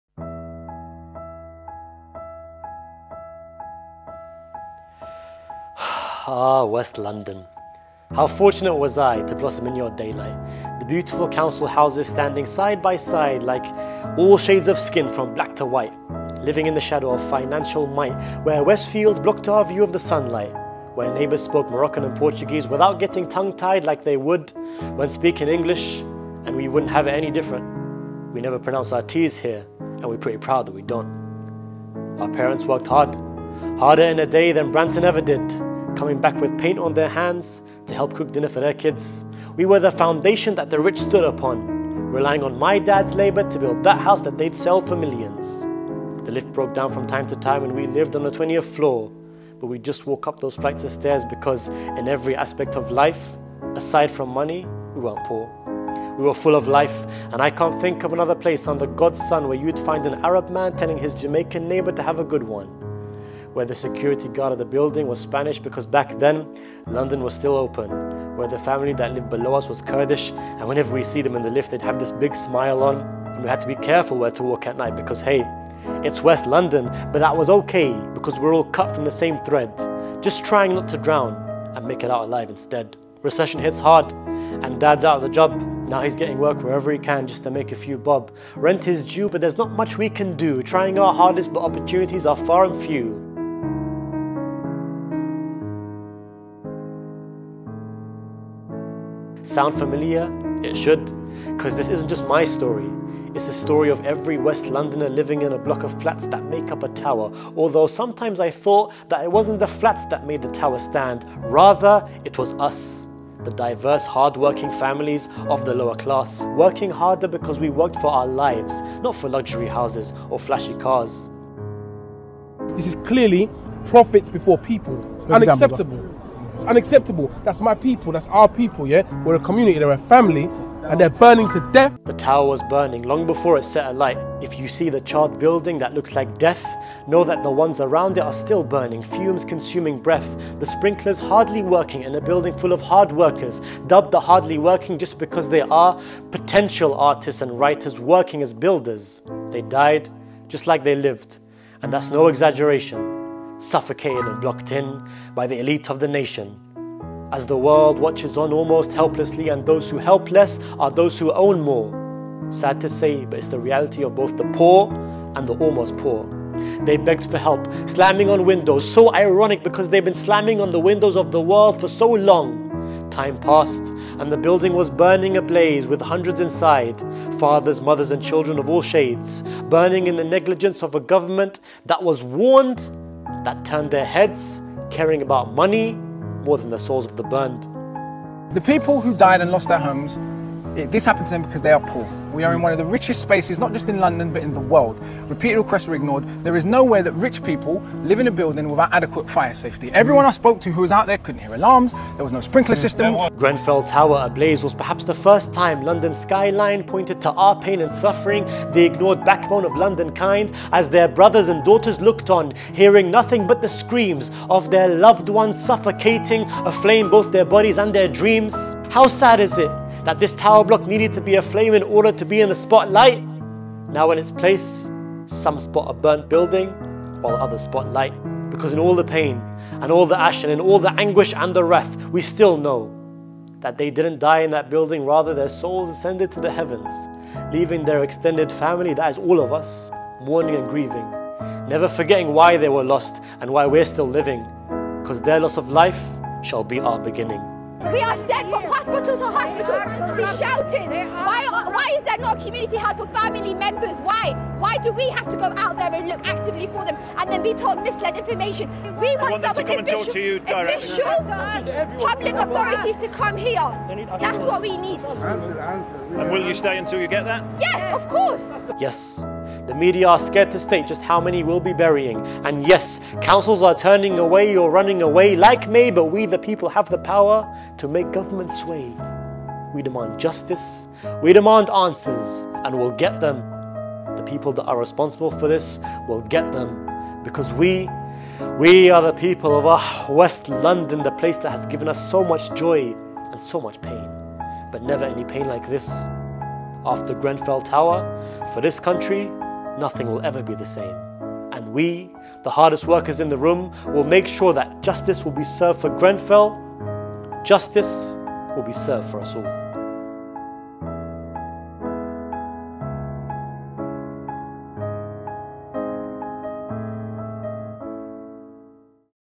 Burnt Skyline || Grenfell Tower Tribute || Spoken Word
A spoken word tribute to the West London I grew up in and the fallen of GrenfellTower.